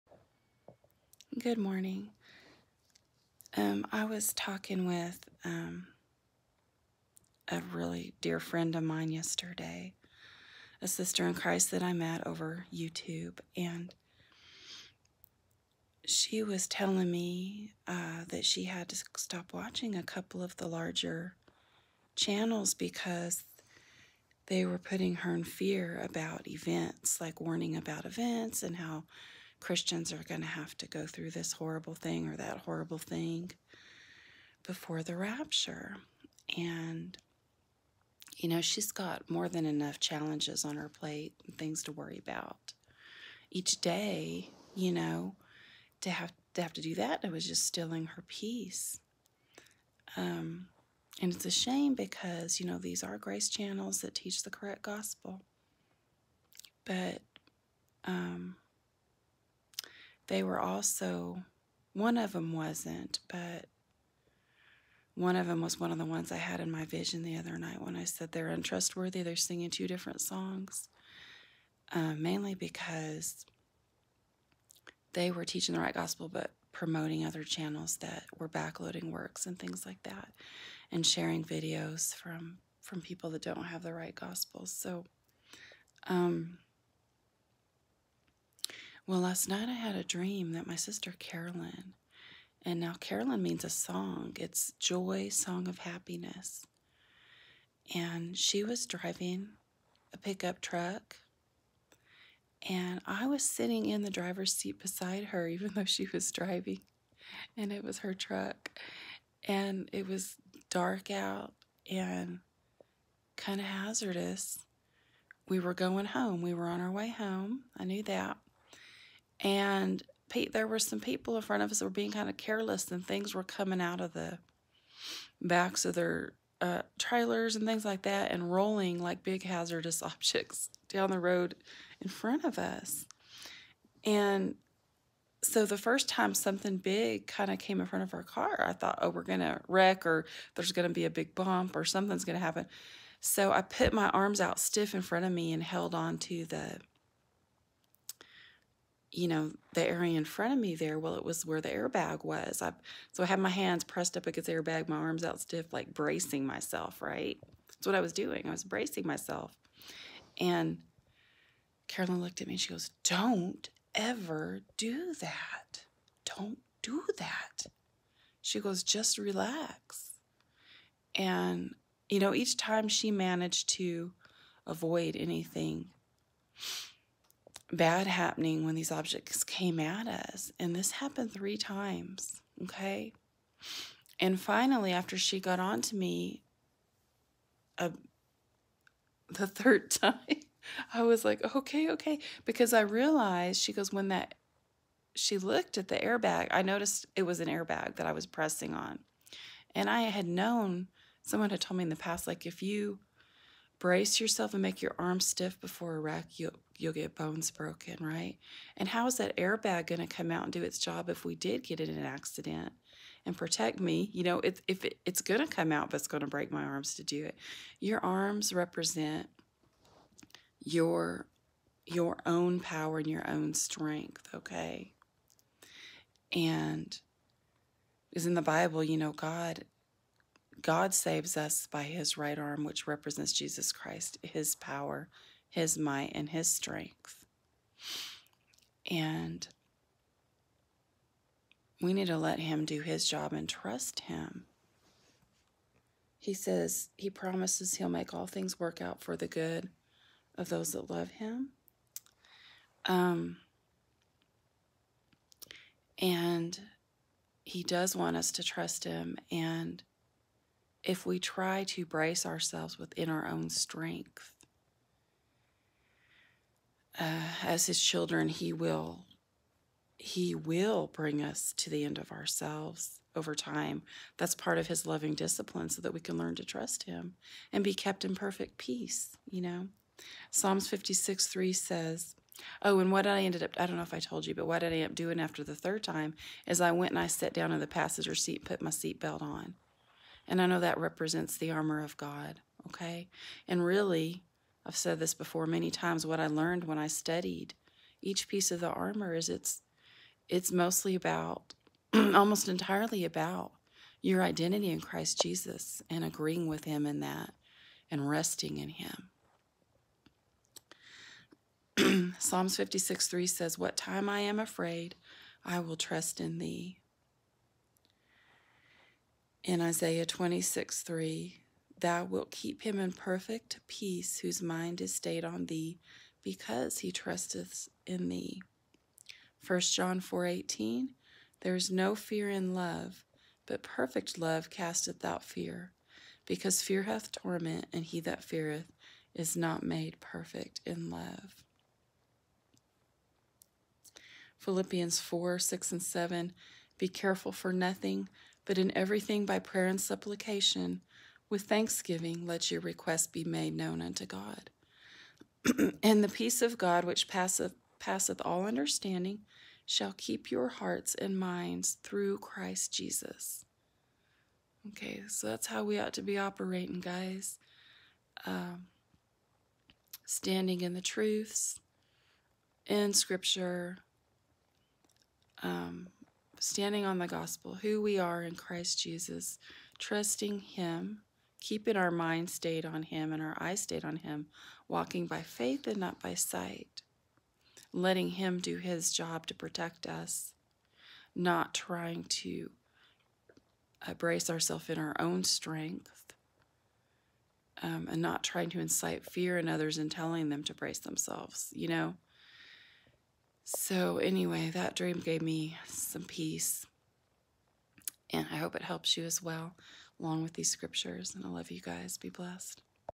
If you BRACE YOURSELF, your arms will be broken. Trust in Jesus. He’s got ya.
Dana Coverstone
Brace Yourself
God’s response to his message and those sharing it.